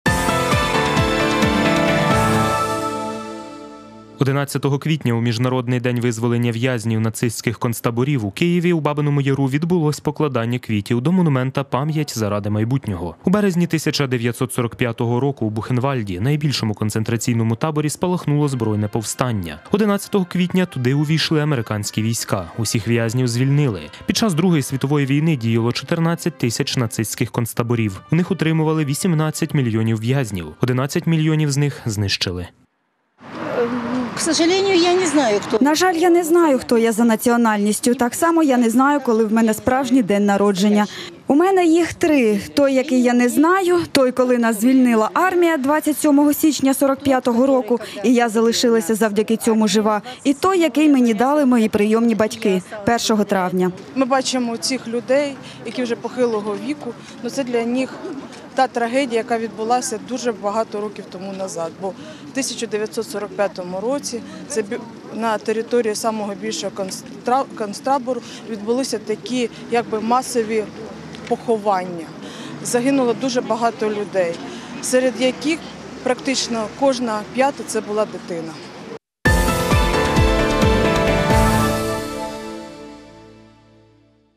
У Києві вшанували пам’ять в’язнів нацистських концтаборів
11 квітня, у міжнародний день визволення в'язнів нацистських концтаборів в Києві, в Бабиному Яру, відбулось покладання квітів до монумента "Пам'ять заради майбутнього". У березні 1945 року в Бухенвальді - найбільшому концентраційному таборі, спалахнуло збройне повстання. 11 квітня туди увійшли американські війська. Усіх в'язнів звільнили.